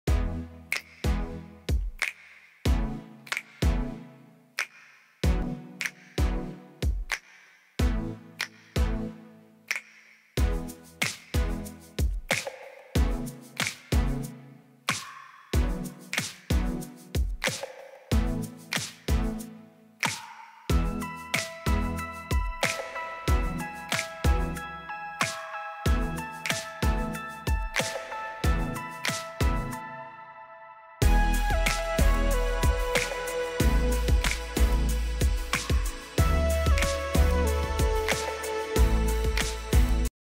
YSL 2020年唇彩新品「時尚印記雪絨唇釉」終於登場！#204、216最軟嫩的雪絨寶寶霧唇絕對必收
YSL時尚印記雪絨唇釉終於要在台灣開賣了！被稱為「黑蓋絲絨唇釉」的它，剛塗抹時帶有水感，接著會變成雪絨霧感，更柔滑滋潤，即使夏天擦也不會覺得過於厚重，尤其#204寶寶粉、216玫瑰甜茶色更是必收美色。詳細介紹請看：